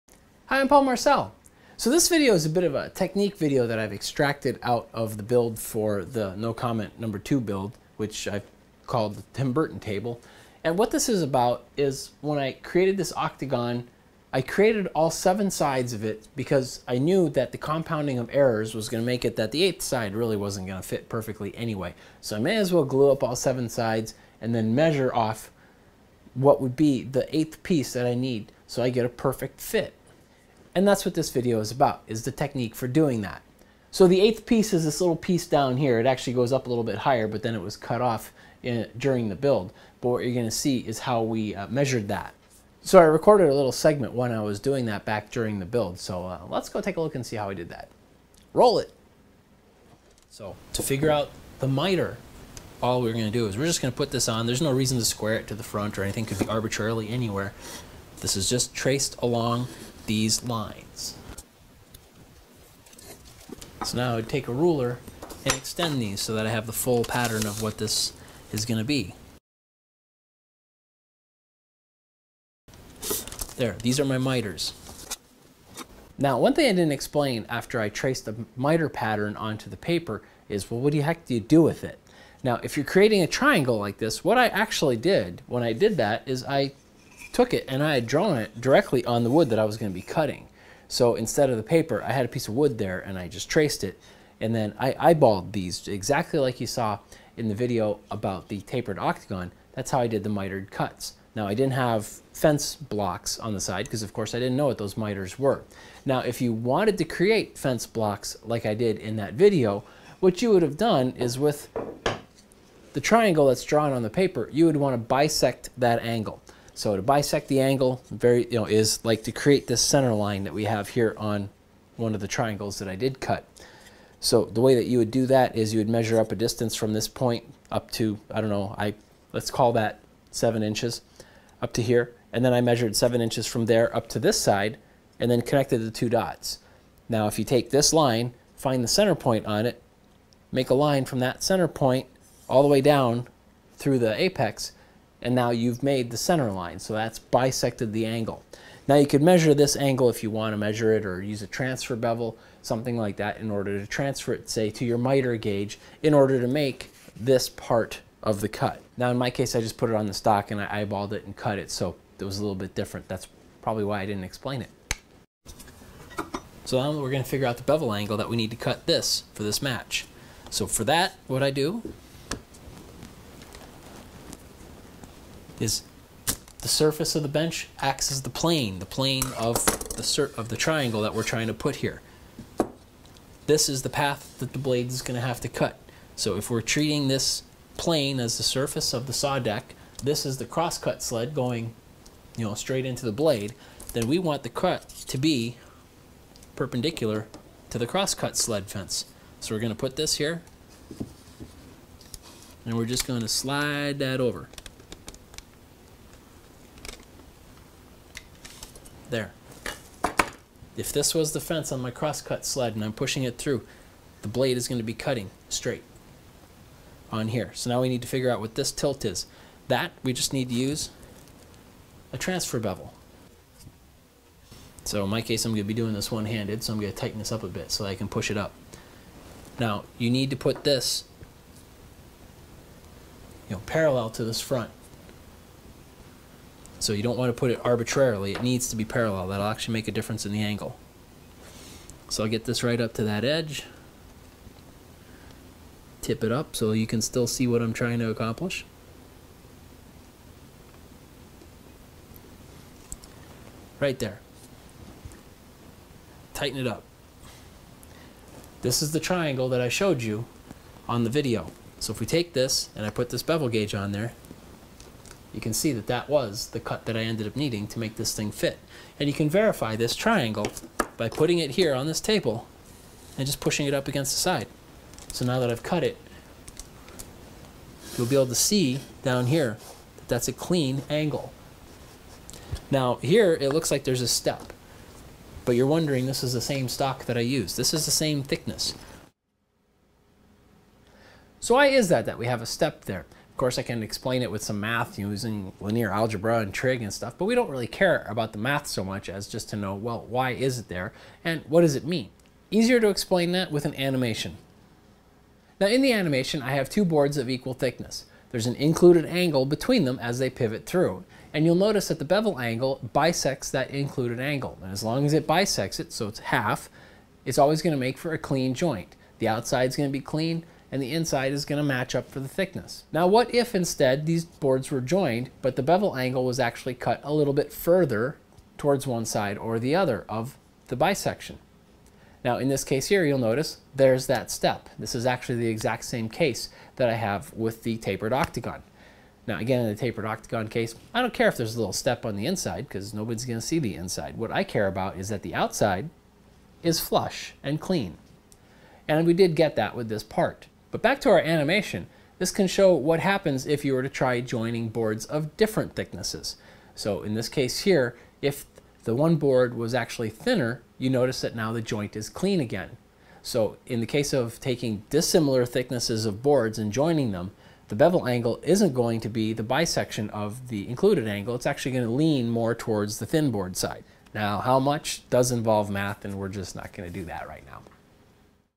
#83 - Technique - Matching Existing Compound Angles
When stacking up compound cuts to make an octagonal cylinder or other multi-faceted object, the minor error in each cut also compounds making the last piece fit less than well.

A better method I used for the tapered octagonal column ("tapered octagon" for short!) was to glue up the first seven facets then measure the exact part I needed for the eighth.  Matching the miter angles of the part is easy, but usually there's confusion on how to measure the associated bevel angles.  This short video (no, really! under 7 minutes!) shows how I did the measurement.

I'll also discuss an animation at the end that shows a bit of how the bevel angle relates to the included angle and stock thickness.

No math is used or harmed in this video; safe for all ages :)